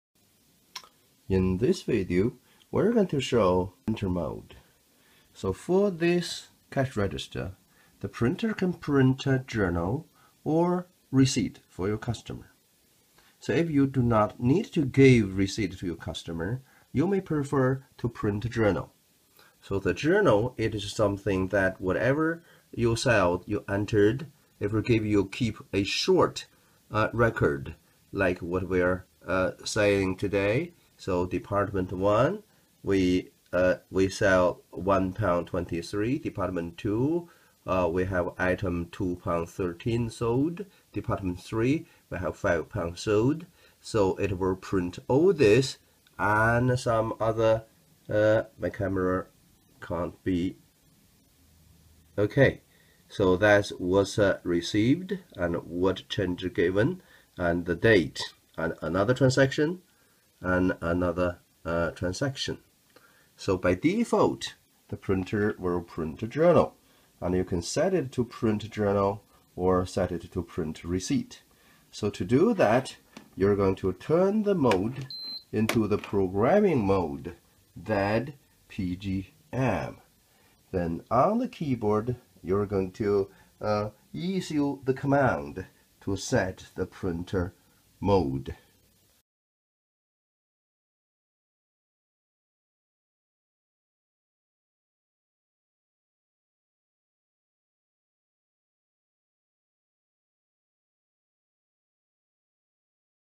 Sharp XE-A102 Cash Register: Printer Modes: Journal or Receipts - which to use
Sharp XE-A102 Cash Register: Printer Modes: Journal or Receipts:

Journal: is the concise selling records for the business to keep;
Receipts: are printed for customers.

You can decide which printer mode you like for your business.

Your comments, suggestions and subscription will be highly appreciated. Thank you.